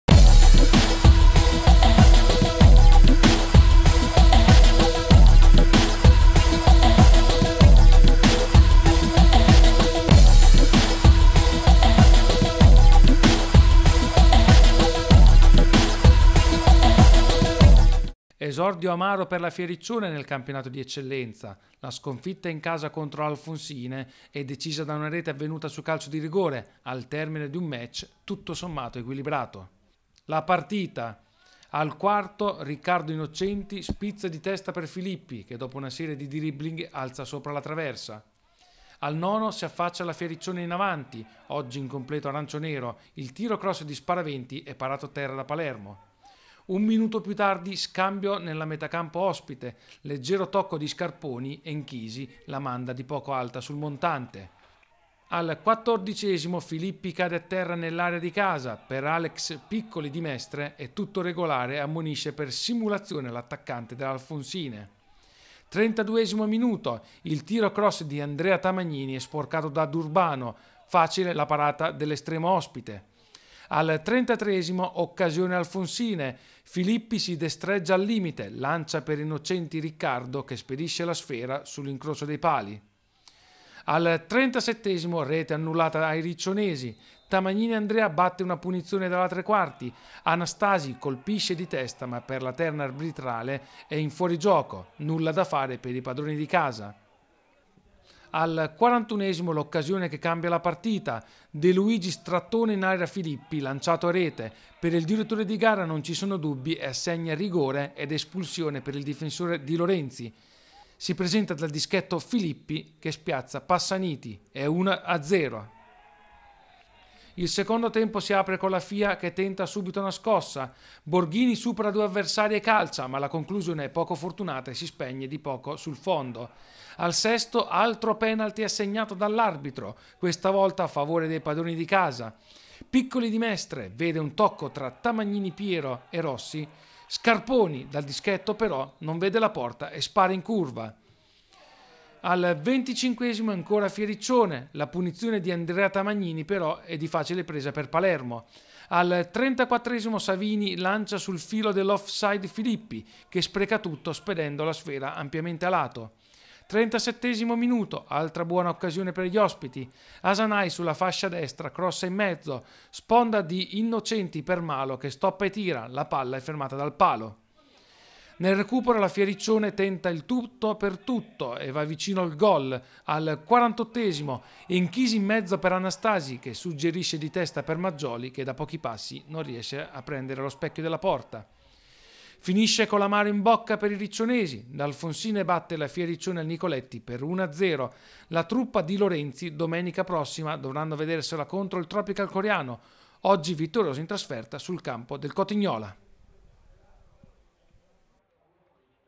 Icaro Sport. Fya Riccione-Alfonsine 0-1, il servizio.
Il servizio della gara della prima giornata di Eccellenza girone C.